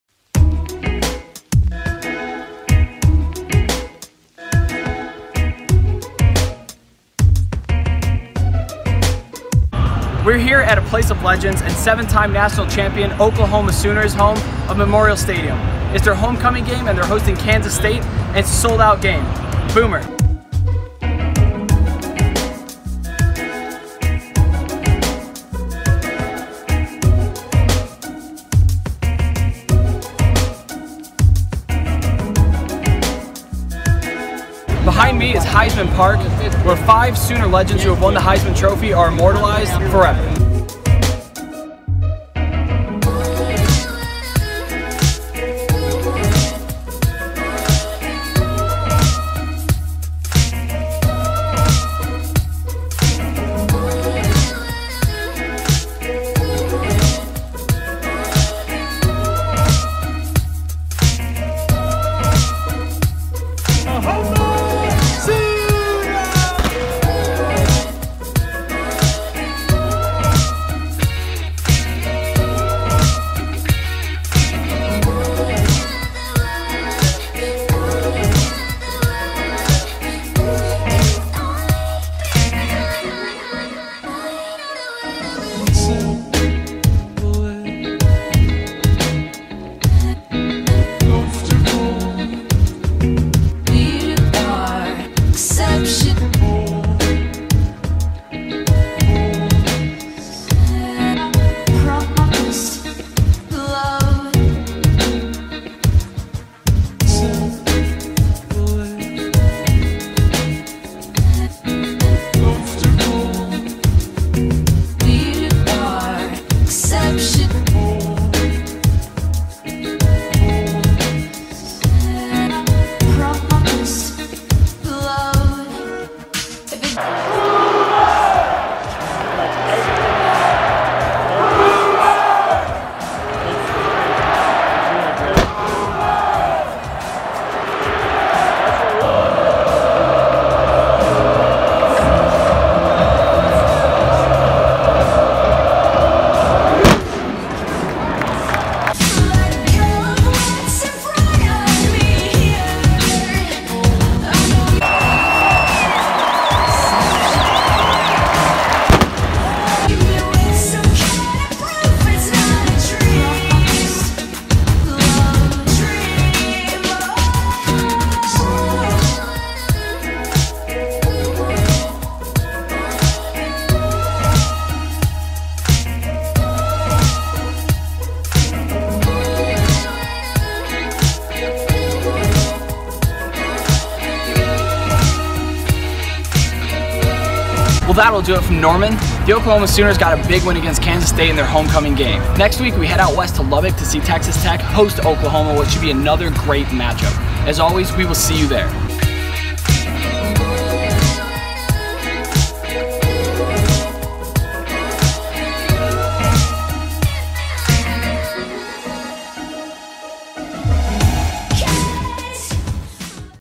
Road to CFB | Oklahoma Game Day | S1E12
The Oklahoma Sooners are all about tradition and history. With 6 Heisman winners and 7 National titles, it is one of the most storied programs in college football.

I check in with the Rufneks and Sooner Men, two spirit groups on campus; meet Boomer, one of the school mascots, and visit Heisman Park and the Walk of Champions. All of that, and it's not even game time yet! The Sooners rolled Kansas State in their homecoming game with not one, but TWO scores of over 80 yards. Electric Heisman contender Kyler Murray puts on a show.

Thanks to partners
- Tailgate Nation
- Thrive Fantasy Football
- Sportseprenuer.com